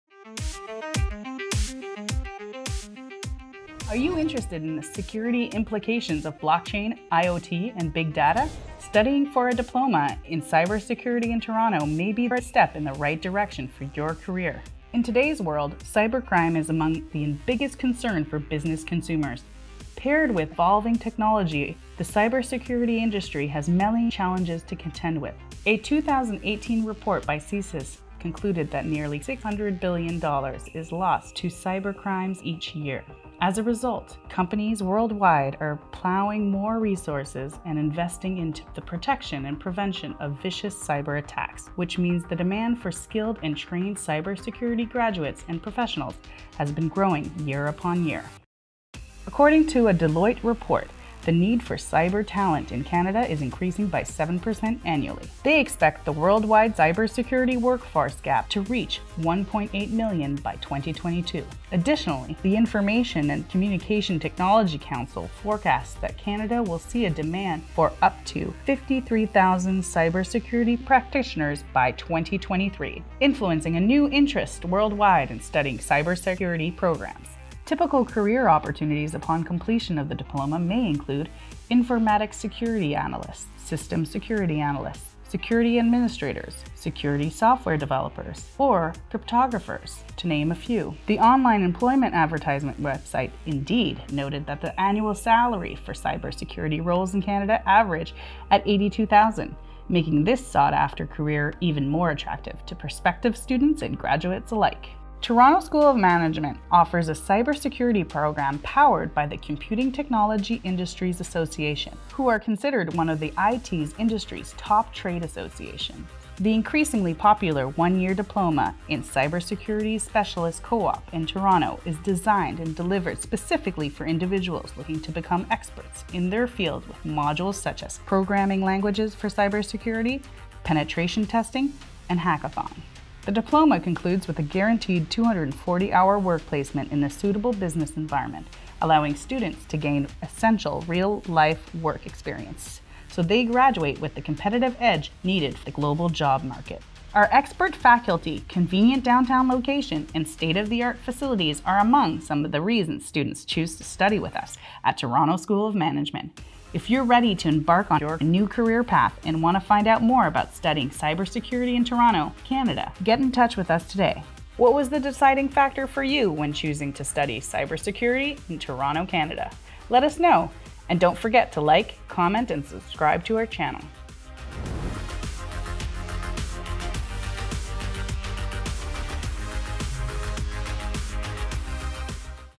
Looking for a career in cybersecurity? Study with us in Toronto, Canada | TSoM
For individuals looking to become experts in the cyber security industry, Toronto School of Management offers the Diploma in Cyber security Specialist Co-op Diploma.

Powered by the Computing Technology Industry Association (CompTIA), the diploma offers students the most relevant and practical knowledge in the cyber security field. Our links with industry partners enable you to get the most out of your program and explore potential career paths.

Throughout the diploma, you will learn to assess risk and develop security policies to protect an organisation’s network and data. You’ll also develop your skills in Penetration Testing, responding to security alerts and managing computer communication systems.

Whilst studying, you’ll to explore the vibrant city of Toronto, where Toronto School of Management is based. With the assistance of expert faculty and small class sizes, you’ll have all the personalised support you need, enabling you to succeed and pursue your dream career.

Once you graduate, you can expect high salaries, exciting challenges and an abundance of work opportunities! Apply today!

Find out more about the Diploma in Cyber security Specialist Co-op Diploma, visit the link here:

Alternatively, if you’d like to find out more about Toronto School of Management, you can visit our